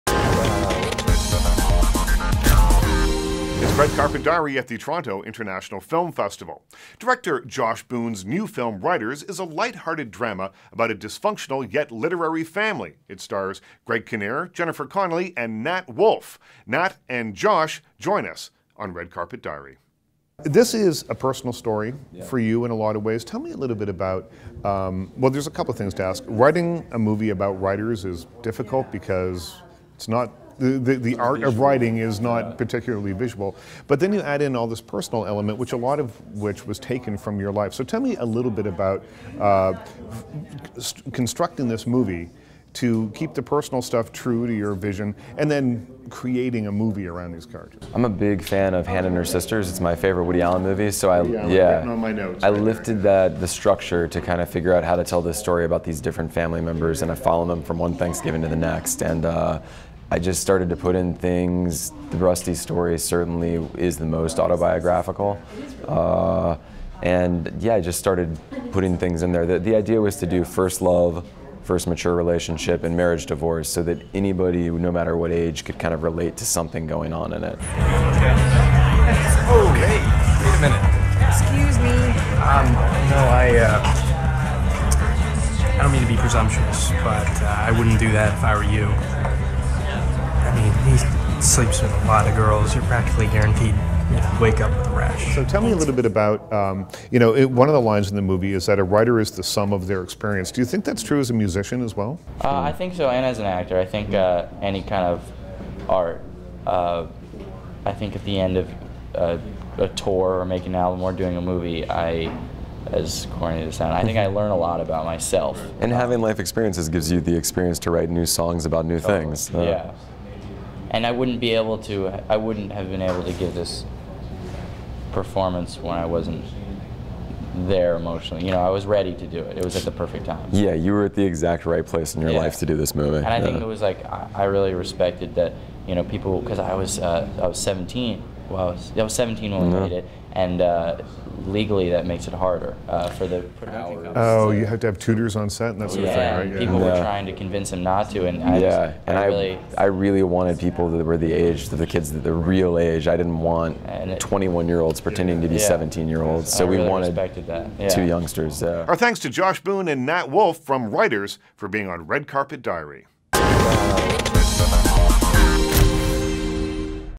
Writer Nat Wolf and Director Josh Boone of Writers at TIFF 2012
Writer Nat Wolf and Director Josh Boone sat down with Richard Crouse to talk about their new film Writers. The story of an acclaimed writer, his ex-wife, and their teenaged children coming to terms with the complexities of love in all its forms over the course of one tumultuous year.Distributed by OneLoad.com